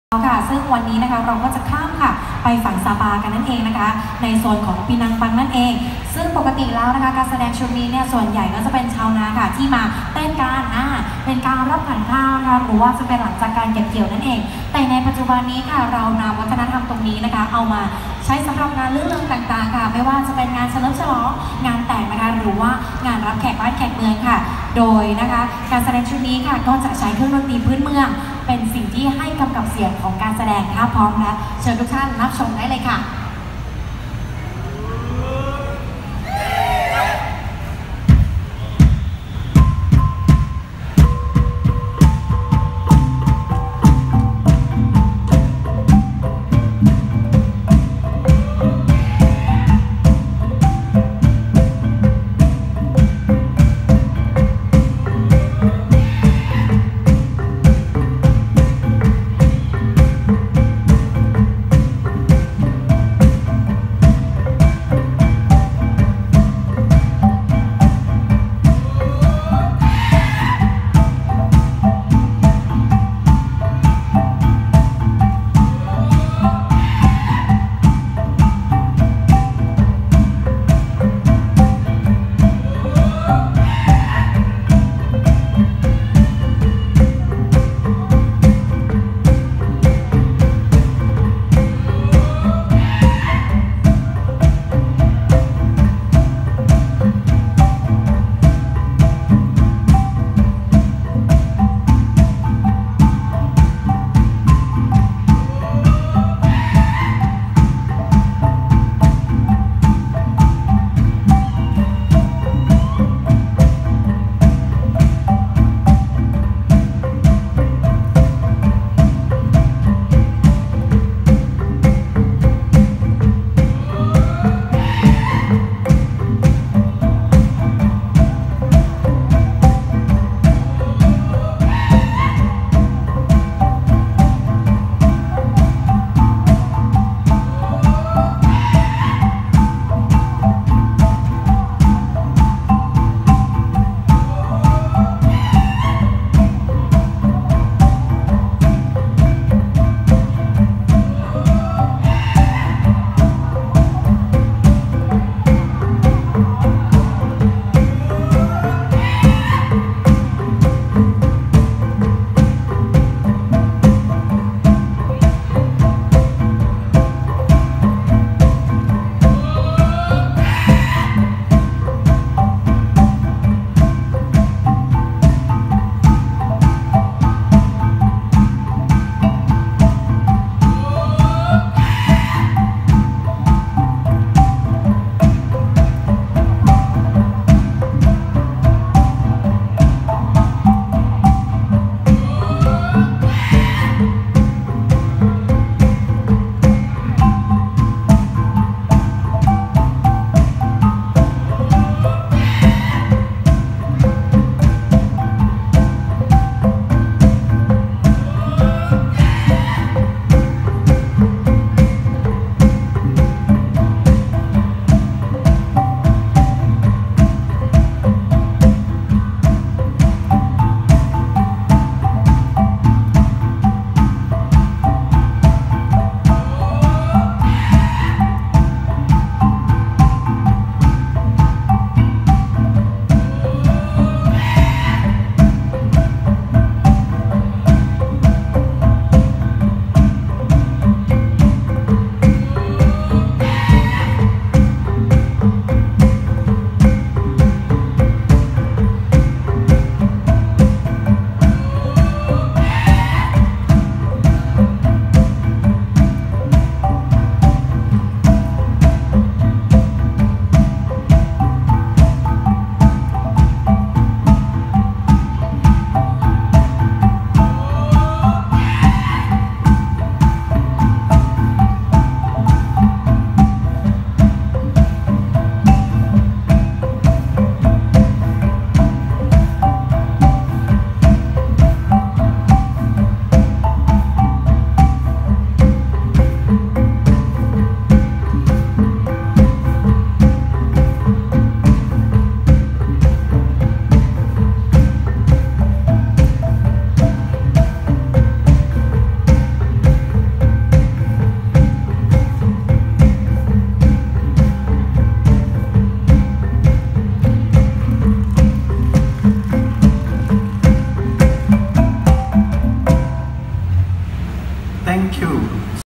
การแสดงจากประเทศมาเลเซีย งาน Malaysia Fest 2019 Central World
การแสดงจากประเทศมาเลเซีย งาน Malaysia Fest 2019 Central World วันศุกร์ที่21มิถุนายน2562.